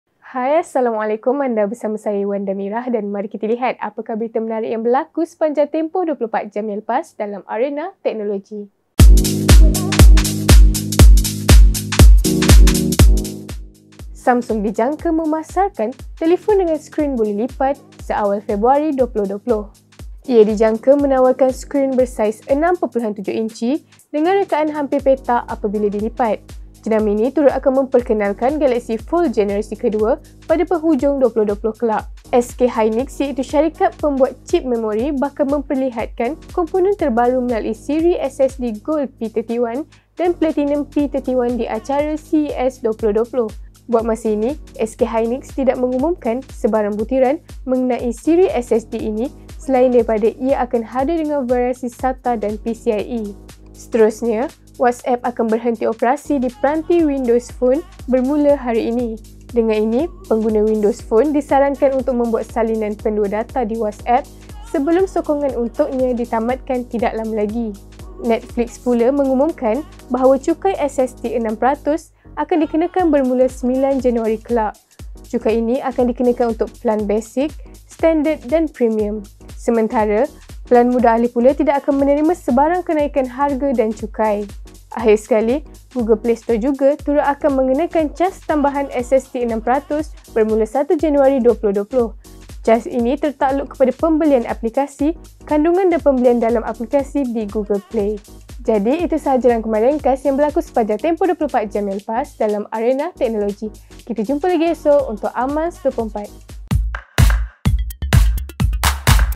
Amanz24 - Galaxy Fold Generasi Kedua Seawal 2020, WhatsApp Hentikan Operasi Peranti Windows Phone
Samsung dijangka memasarkan telefon dengan skrin boleh-lipat seawal Februari 2020. Ia dijangka menawarkan skrin bersaiz 6.7-inci dengan rekaan hampir petak apabila dilipat. Jenama ini turut akan memperkenalkan Galaxy Fold generasi kedua pada penghujung 2020 kelak.

SK Hynix iaitu syarikat pembuat cip memori bakal memperlihatkan komponen terbaru melalui siri SSD Gold P31 dan Platinum P31 di acara CES 2020. Buat masa ini, SK Hynix tidak mengumumkan sebarang butiran mengenai siri SSD ini, selain daripada ia akan hadir dengan variasi SATA dan PCIe.

Seterusnya, WhatsApp akan berhenti operasi di peranti Window Phone bermula hari ini. Dengan ini, pengguna Windows Phone disarankan untuk membuat salinan pendua data di WhatsApp, sebelum sokongan untuknya ditamatkan tidak lama lagi.

Netflix pula mengumumkan bahawa cukai SST 6 peratus akan dikenakan bermula 9 Januari kelak. Cukai ini akan dikenakan untuk pelan Basic, Standard dan Premium. Sementara, pelan mudah alih pula tidak akan menerima sebarang kenaikan harga dan cukai.

Akhir sekali, Google Play Store juga turut akan mengenakan caj tambahan SST 6 peratus bermula 1 Januari 2020. Caj ini tertakluk kepada pembelian aplikasi, kandungan dan pembelian dalam-aplikasi di Google Play.